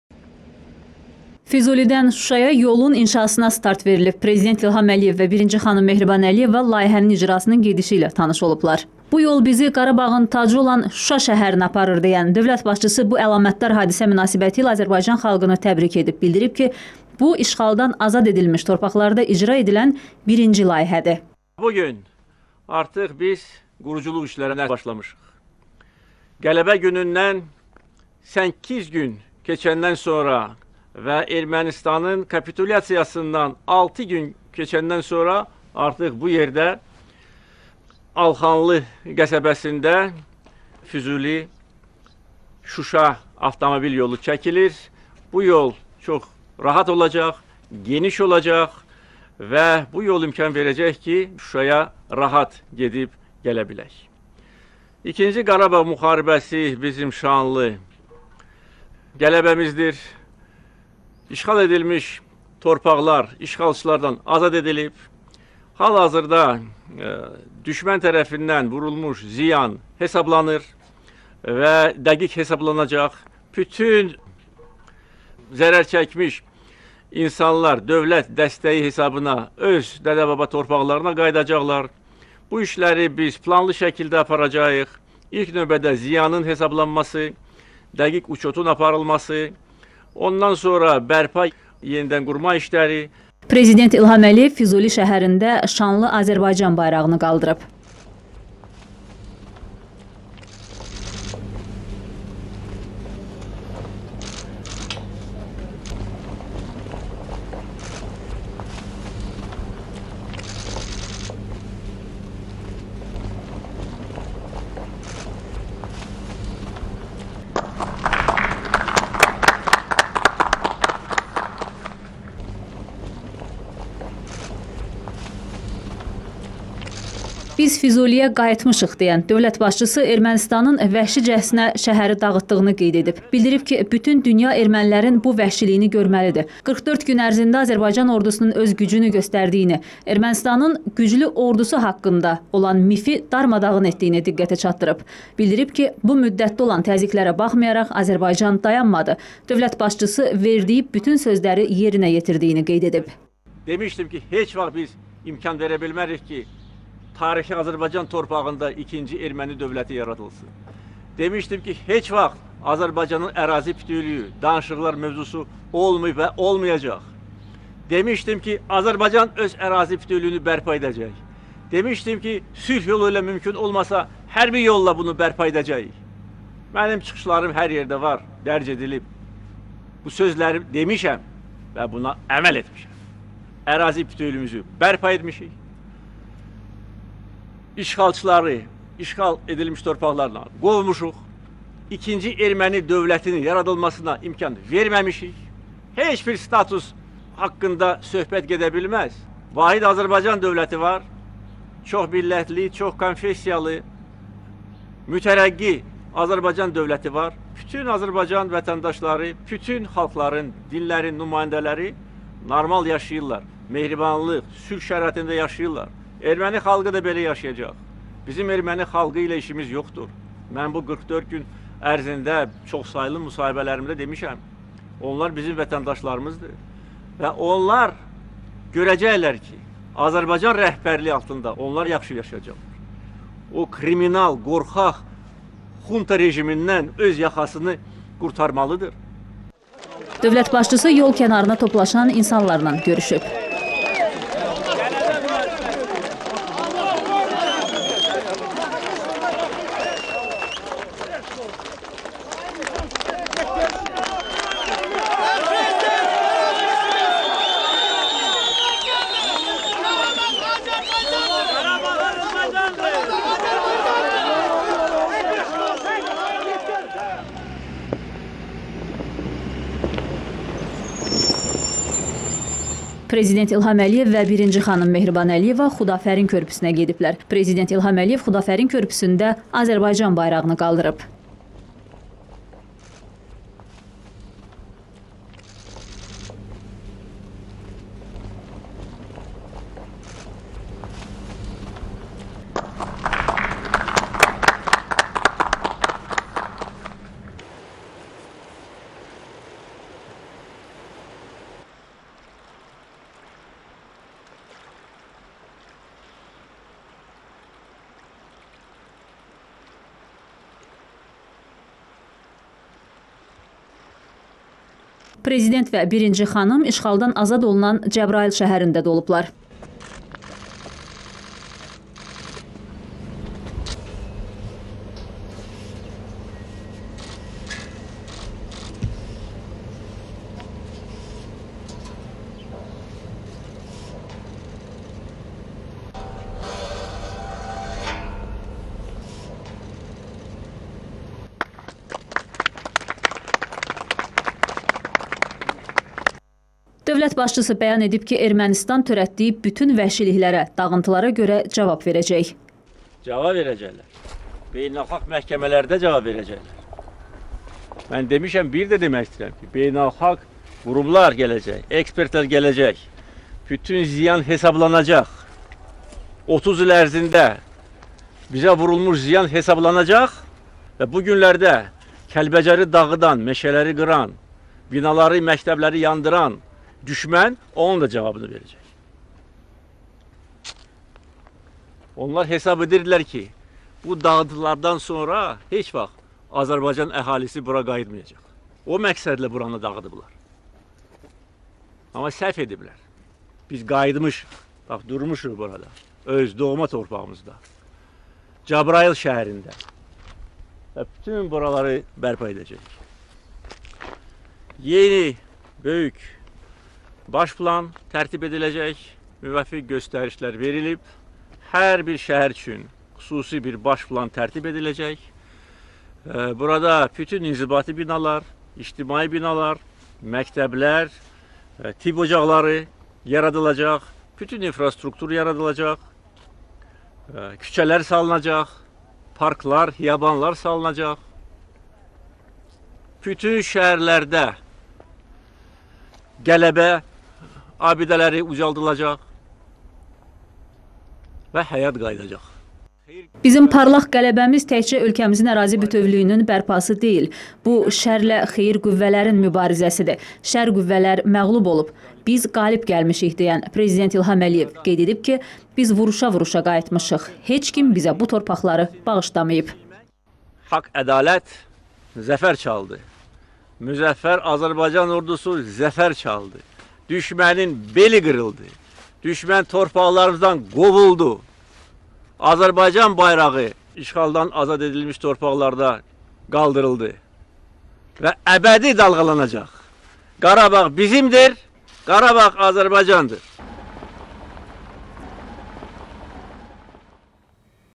PREZİDENT İLHAM ƏLİYEV:“DÜŞMƏN TƏRƏFİNDƏN VURULMUŞ ZİYAN HAZIRDA HESABLANIR”
Füzulidən Şuşaya youn inşasına start verilib. Prezident İlham Əliyev və birinci xanım Mehriban Əliyeva layihənin icrasının gedişi ilə tanış olublar. “Bu yol bizi Qarabağın tacı olan Şuşa şəhərinə aparır.”- deyən dövlət başçısı bu əlamətdar hadisə münasibətilə Azərbaycan xalqını təbrik edib. Bildirib ki, bu, işğaldan azad edilmiş torpaqlarda icra edilən birinci layihədir.
Prezident İlham Əliyev Füzuli şəhərində şanlı Azərbaycan bayrağını qaldırıb.
“Biz Füzuliyə qayıtmışıq” – deyən dövlət başçısı Ermənistanın vəhşicəsinə şəhəri dağıtdığını qeyd edib. Bildirib ki, bütün dünya ermənilərin bu vəhşiliyini görməlidir. 44 gün ərzində Azərbaycan ordusunun öz gücünü göstərdiyini, Ermənstanın gücü ordusu haqqında mifi darmadağın etdiyimizi diqqətə çatdıran prezident İlham Əliyev deyib ki, bu müddətdə olan təzyiqlərə baxmayraq Azərbaycanın dayanmadı. Dövlət başçısı verdiyi bütün sözləri yerinə yetirdiyini bildirib.
Dövlət başçısı yol kənarına toplaşan insanlarla görüşüb.
Prezident İlham Əliyev və birinci xanım Mehriban Əliyeva Xudafərin körpüsünə gediblər. Prezident İlham Əliyev Xudafərin körpüsündə şanlı Azərbaycan bayrağını qaldırıb.
Prezident və birinci xanım Cəbrayıl şəhərində də olublar.
Dövlət başçısı bəyan edib ki, Ermənistan törətdiyi bütün vəhşiliklərə, dağıntılara görə cavab verəcək.
“Bizim parlaq Qələbəmiz təkcə ölkəmizin ərazi bütövlüyünün bərpası deyil. Bu, şərlə xeyir qüvvələrinin mübarizəsidir. Şər qüvvələri məğlub olub, amma biz qalib gəlmişik.” – deyən prezident İlham Əliyev bildirib ki, biz vuruşa-vuruşa qayıtmışıq. Heç kim bizə bu torpaqları bağışlamayıb.